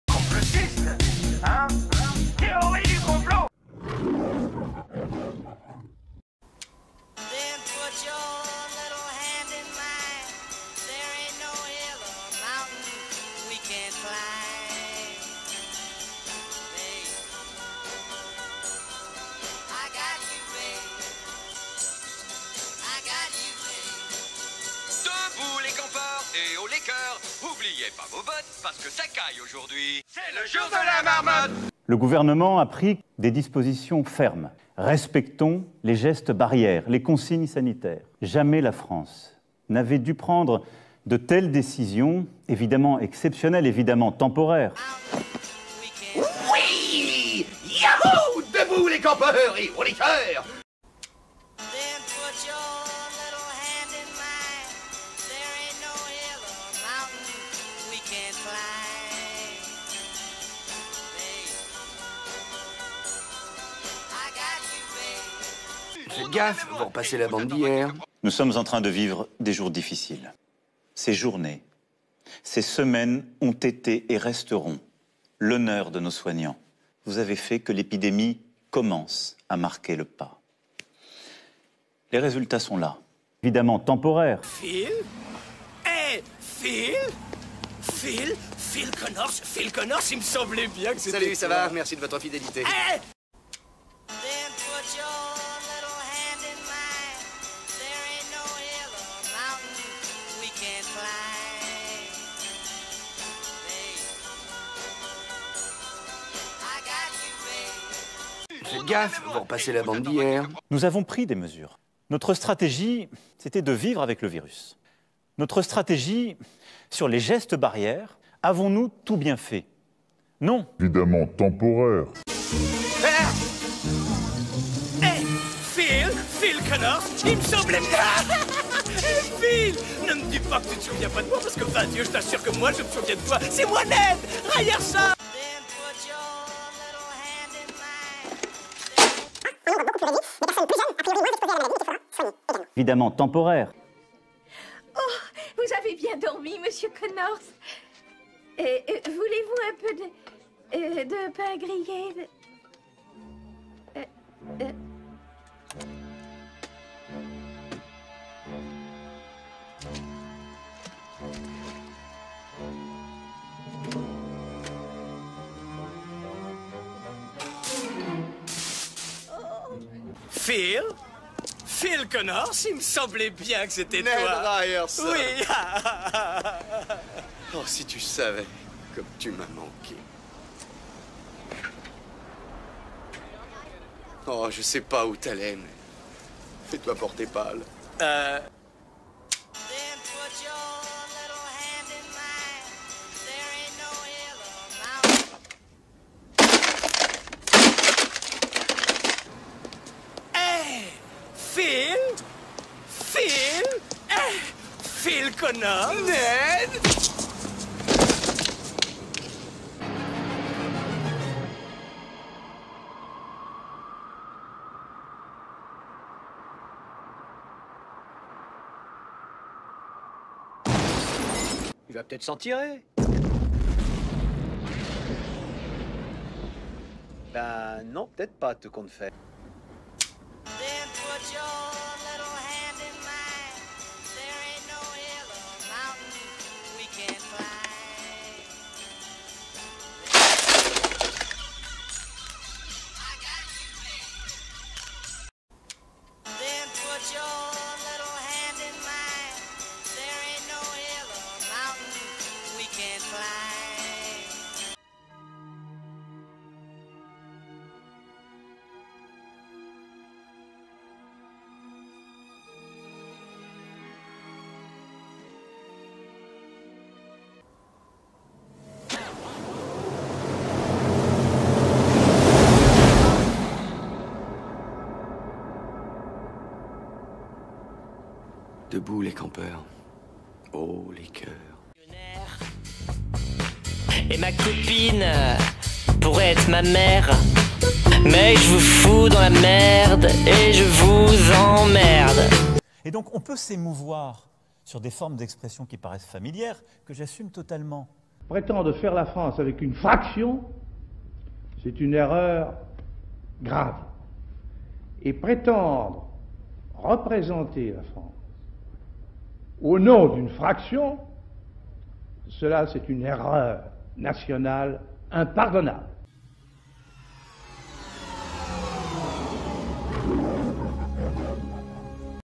L'exemple typique d'un "Jour sans fin" avec des mesures "anti COCO19" sans fin !
Retrouvez une parodie du Film Culte "Un jour sans Fin" en lien avec l'actualité.